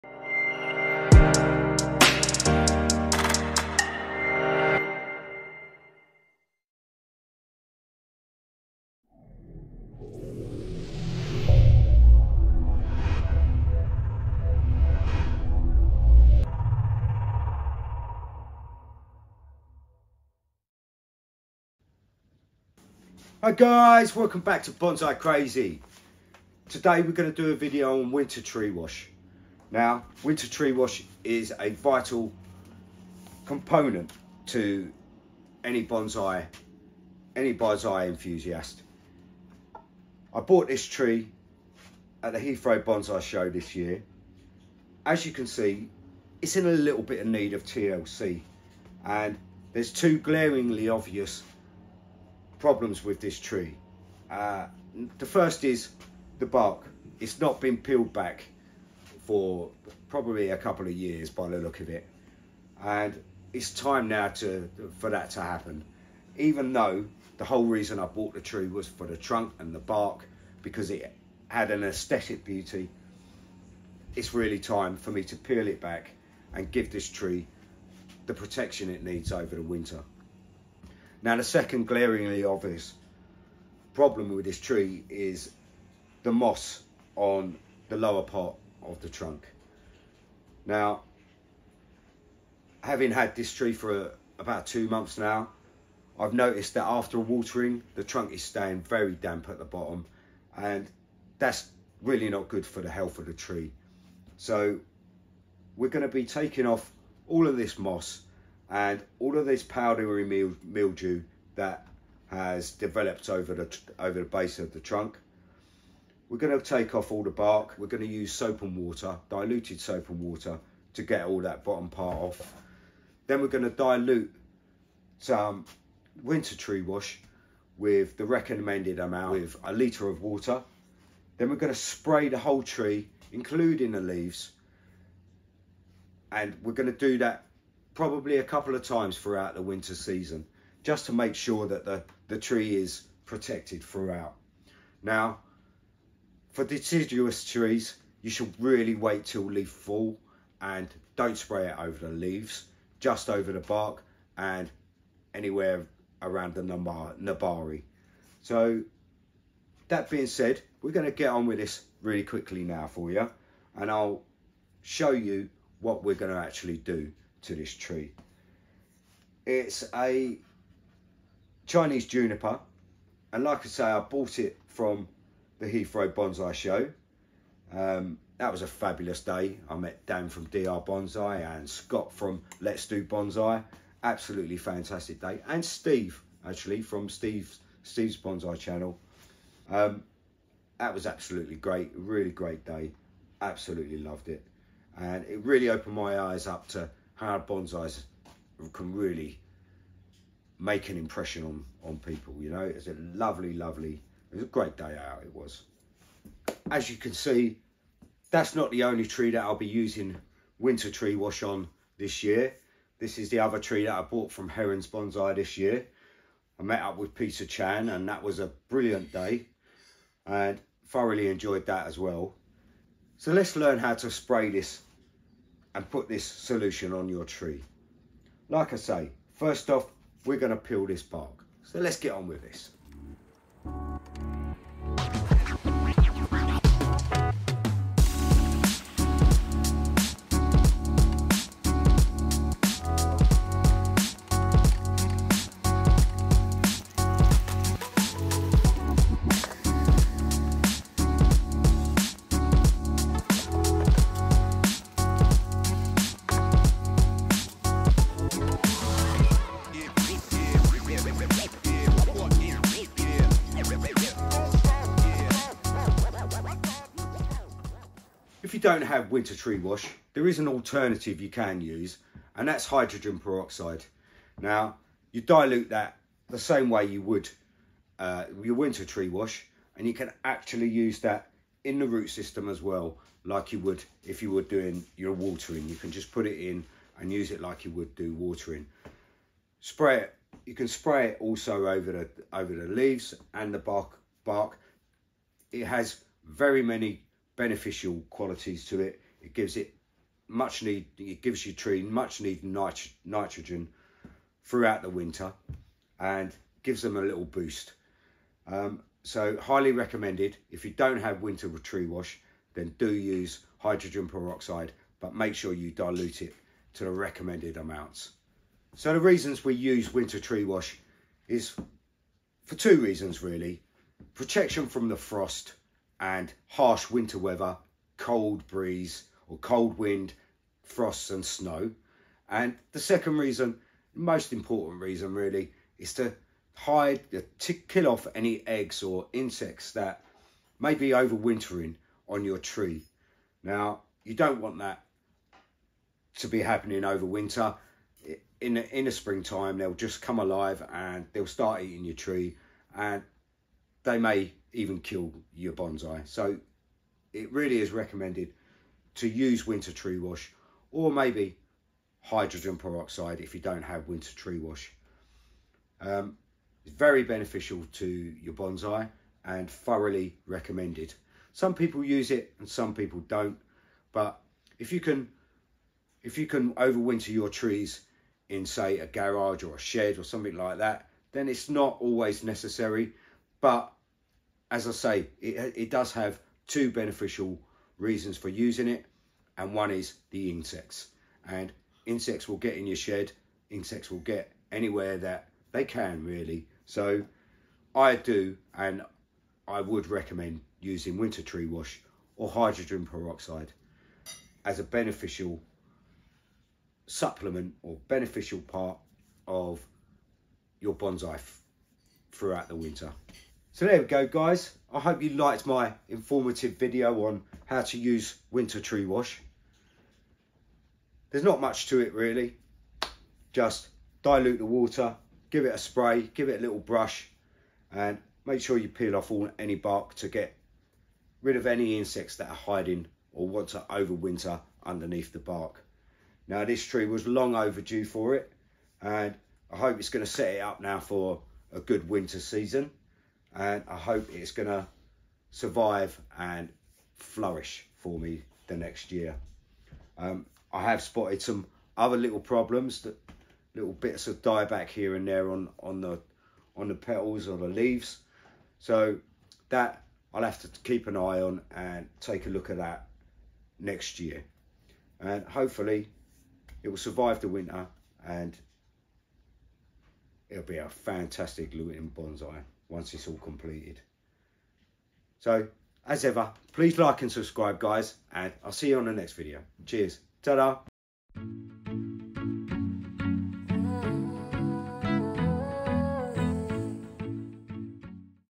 “Winter tree wash for bonsai: protecting and nurturing your miniature masterpieces
Winter tree wash for bonsai, how to apply and use winter tree wash a step by step guide by @Bonsaicrazy